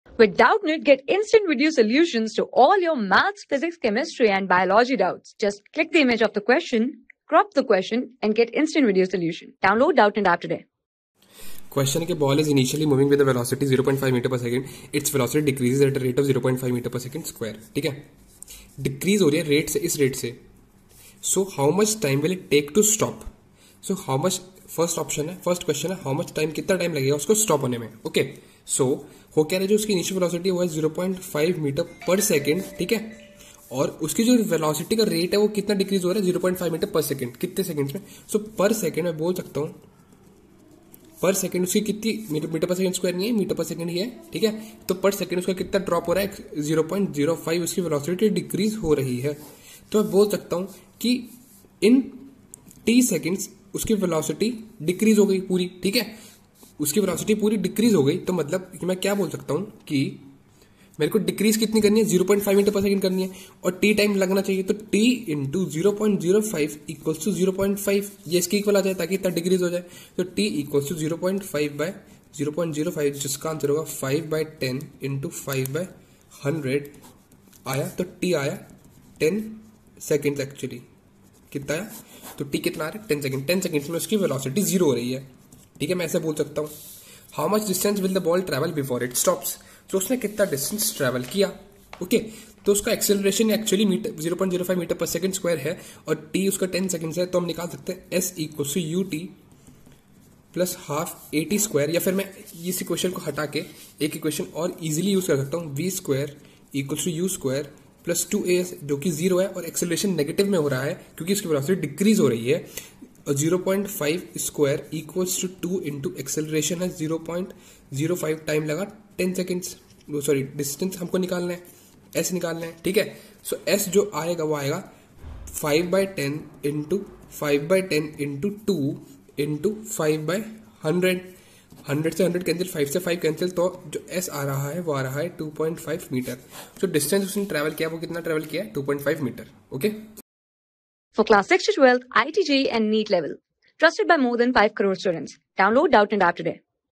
A ball is initially moving with a velocity 0.5 m s^(-1) ms. Its velocity decreases at a rate of ...
A ball is initially moving with a velocity 0.5 m s^(-1) ms. Its velocity decreases at a rate of 0.05 m s^(-2). (a) How much time will it take to stop ? (b) How much distance will the ball travel before it stops ?
Class: 9
Subject: PHYSICS
Chapter: MOTION IN ONE DIMENSION
Board:ICSE

👉 Have Any Query? Ask Us.